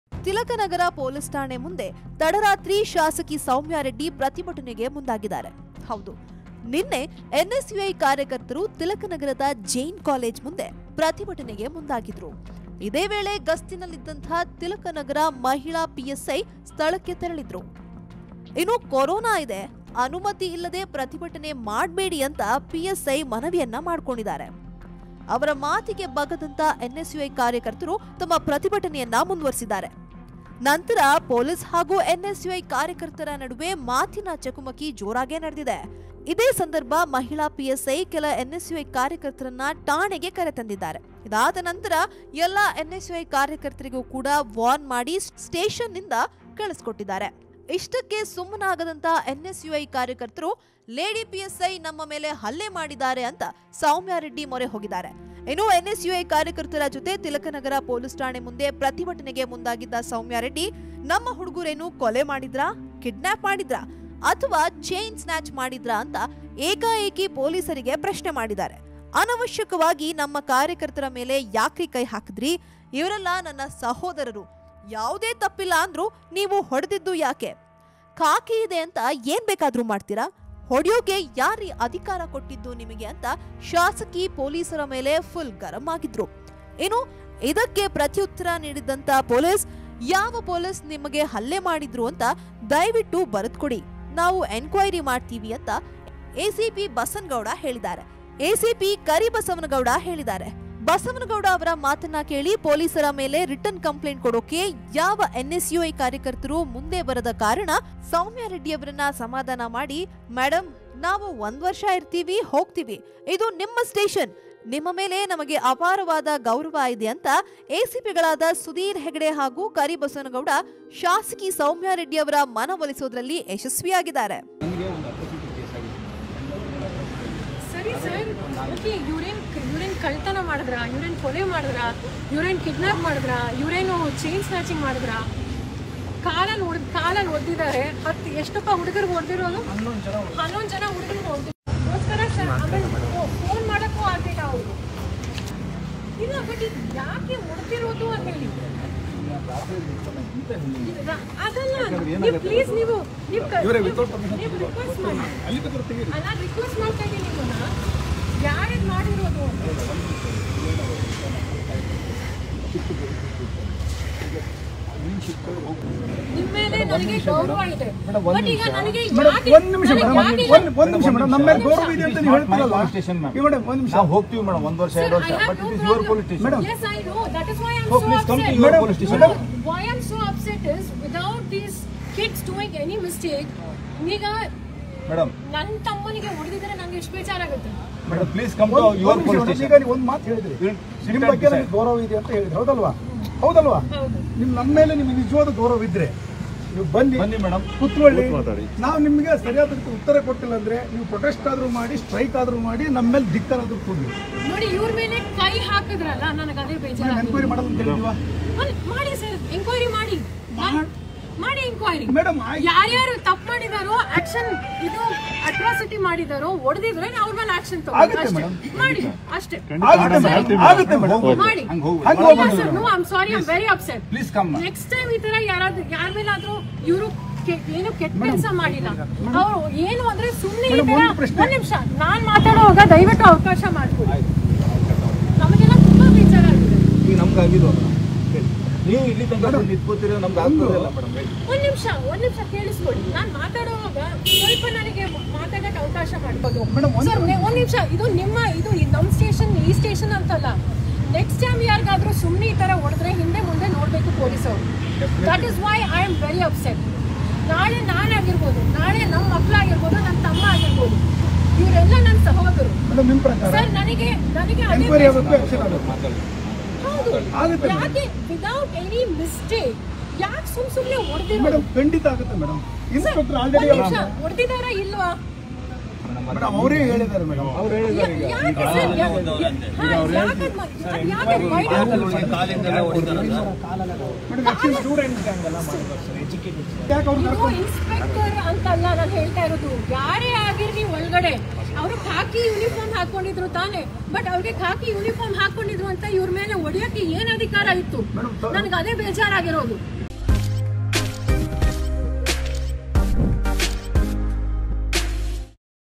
Sowmya Reddy, Thilak Nagar ಪೊಲೀಸರ ಜೊತೆ ಕಿರಿಕ್ | Oneindia Kannada
NSUI activists who do not want to give a return compliant to any police
ACPs Sudheer Hegde and Karibasawanagouda appealed to MLA Soumya Reddy

ಈ ವೇಳೆ ಯಾವ ಪೊಲೀಸರ ಮೇಲೂ ರಿಟನ್ ಕಂಪ್ಲೆಂಟ್ ಕೊಡಲು ಮುಂದೆ ಬಾರದ ಎನ್.ಎಸ್.ಯು.ಐ ಕಾರ್ಯಕರ್ತರು
ಶಾಸಕಿ ಸೌಮ್ಯರೆಡ್ಡಿಯನ್ನ ಕಡೆಗೂ ಸಮಾಧಾನ ಮಾಡಿದ ಎಸಿಪಿಗಳಾದ ಸುಧೀರ್ ಹೆಗ್ಡೆ ಹಾಗೂ ಕರಿಬಸವನಗೌಡ